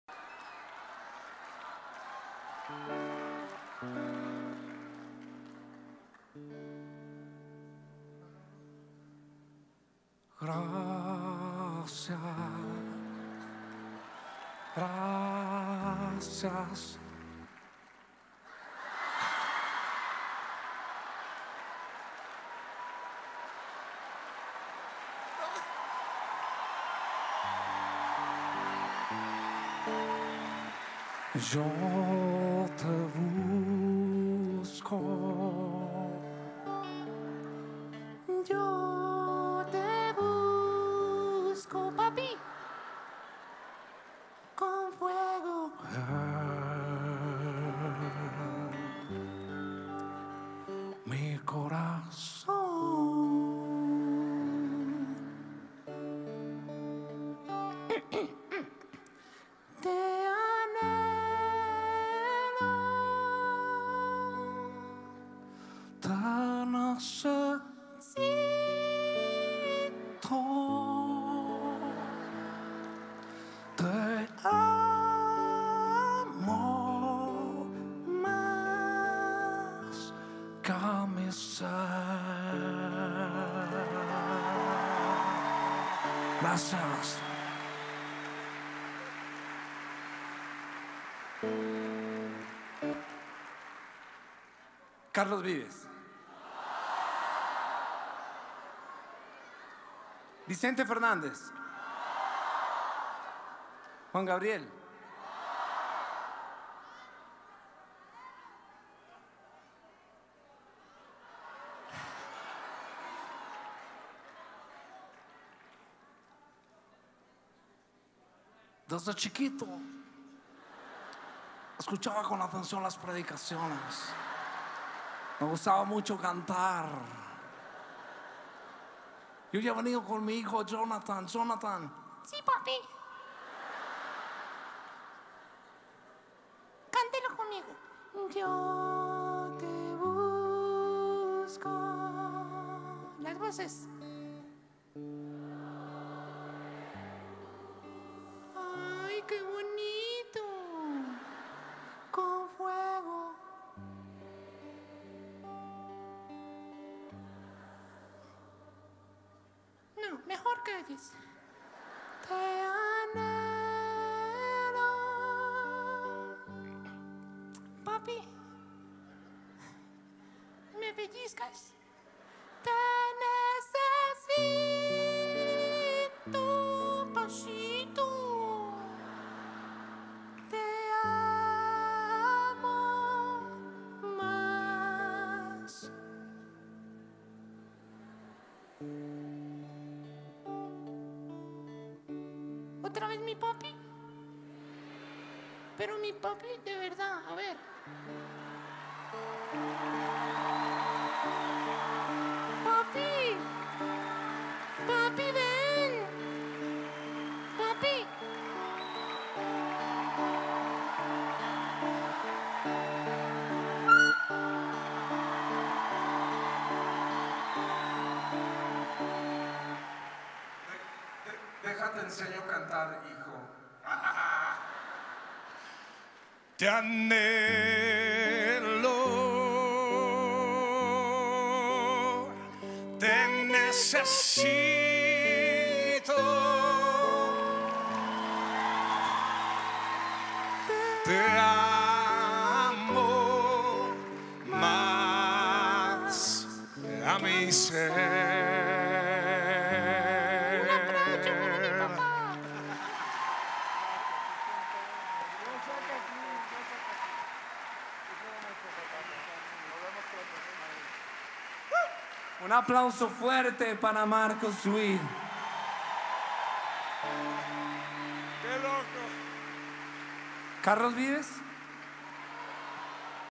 Fernando Ramos imita a Marcos Witt delante de él - G12TV
Pastor Fernando Ramos imitando a Marcos Witt